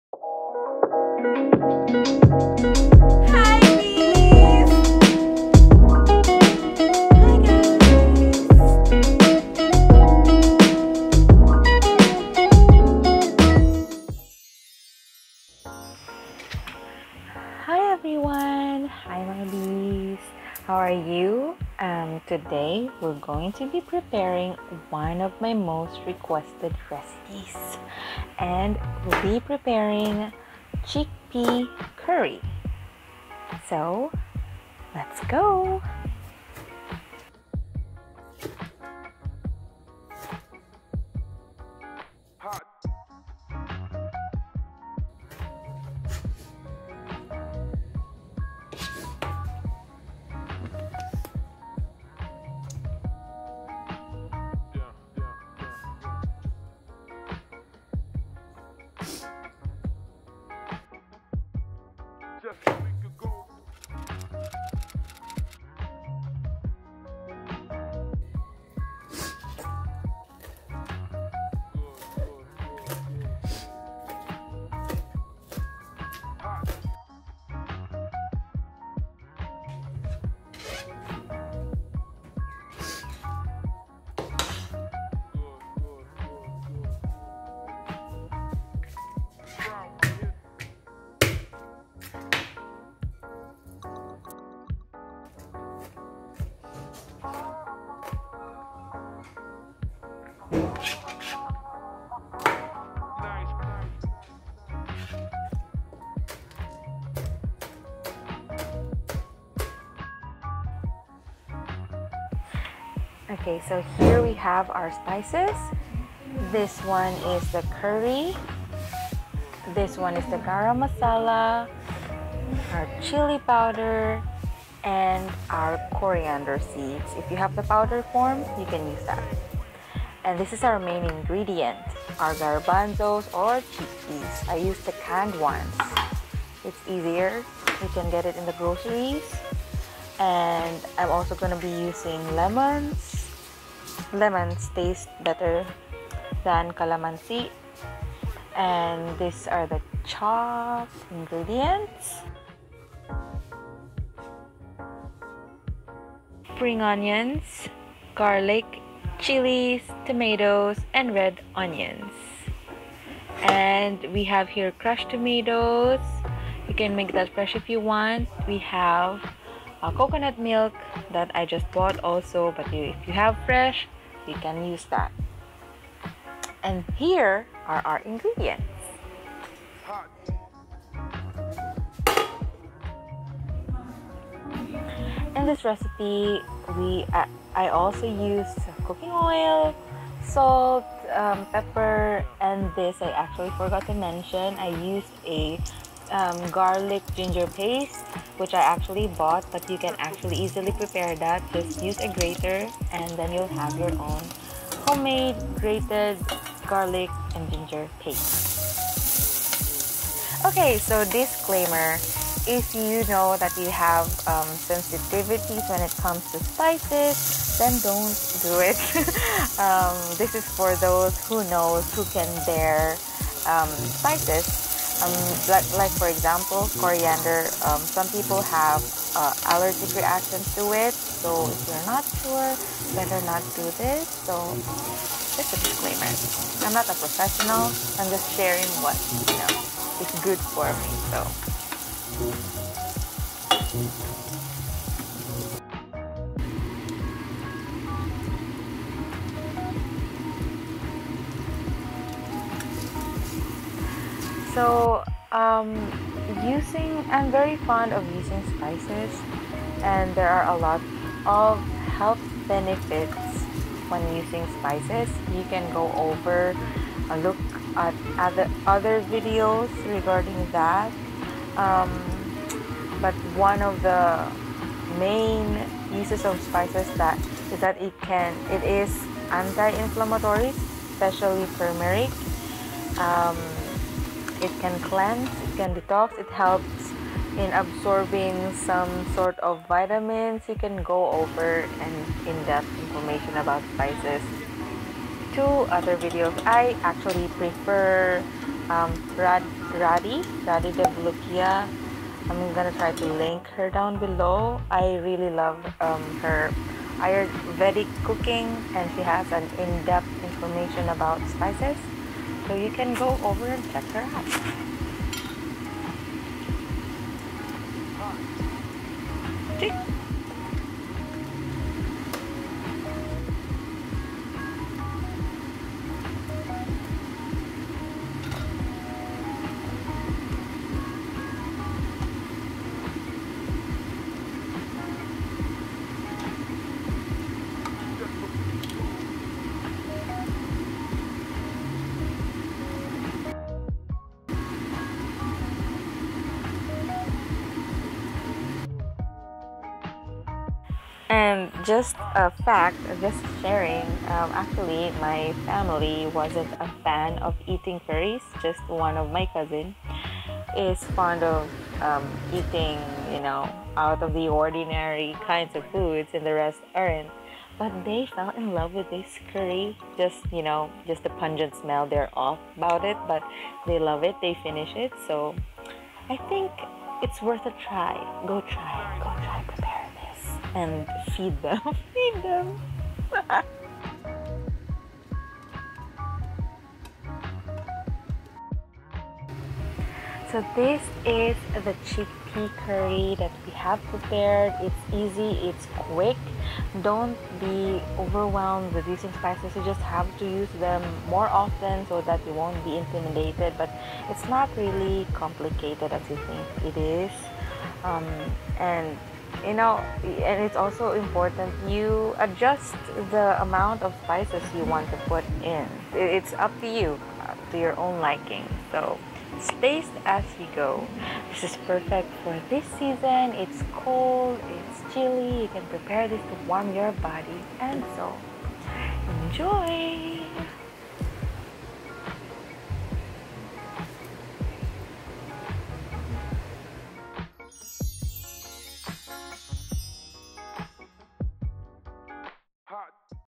Spice Up with Chick Pea Curry!
Join me as I prepare a hot bowl of spice for these cold days!